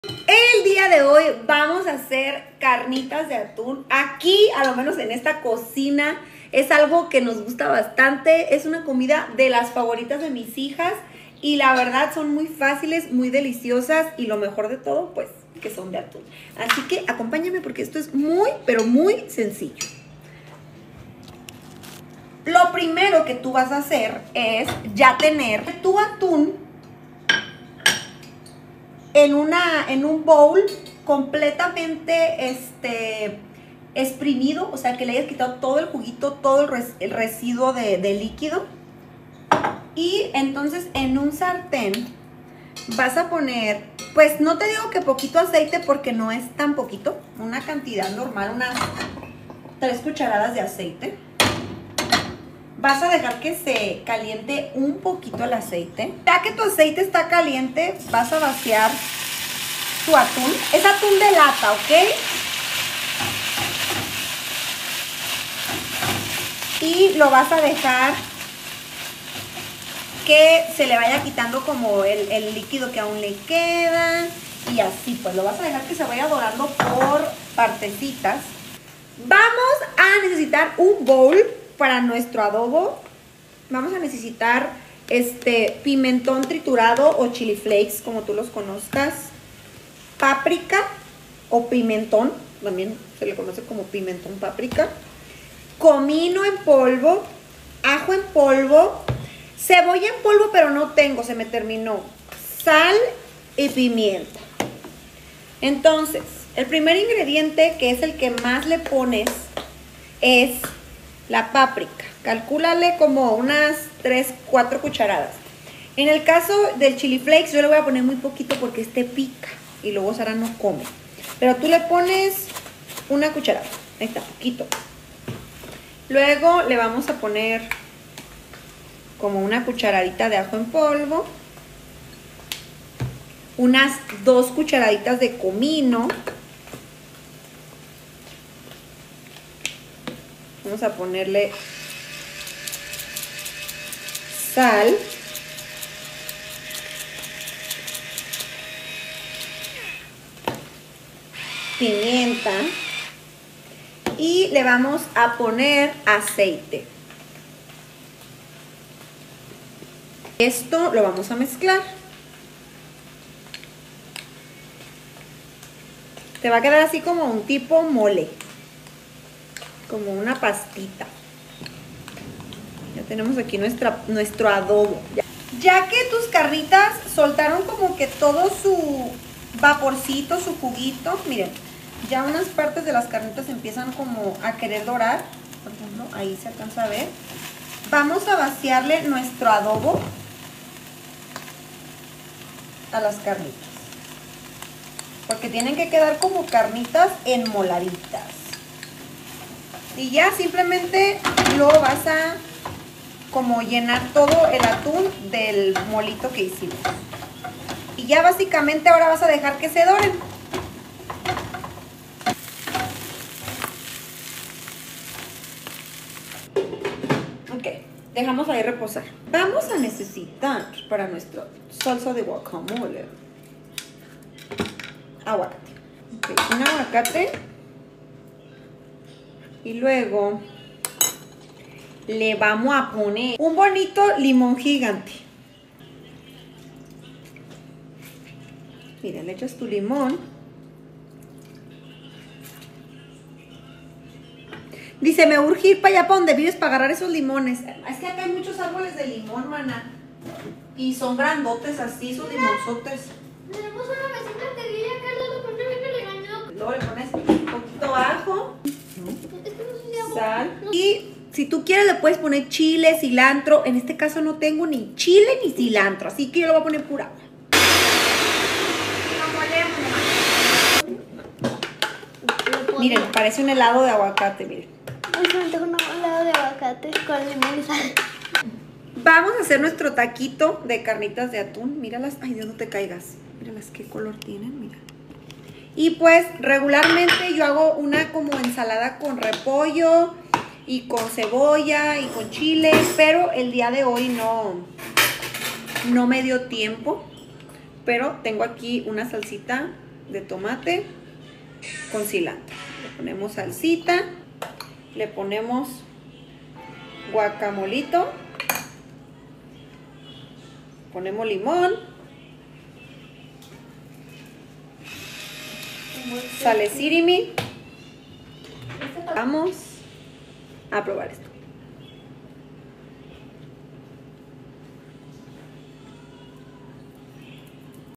DELICIOSAS CARNITAS DE ATÚN😱 *receta completa*
Hoy les traigo esta receta saludable para ese antojo de carnitas! Ideal cuando quieres comer sano y rico Talis prueben hacer esta increíble receta 🤩